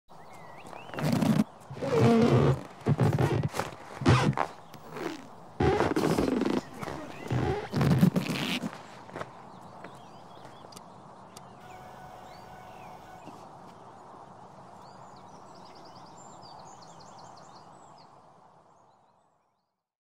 Discovery Sport | Empaca todo lo que necesites
Empaca todo lo necesario para tu próxima aventura, y no dejes nada fuera por falta de espacio.
Con el Discovery Sport NeverStopDiscovering🇲🇽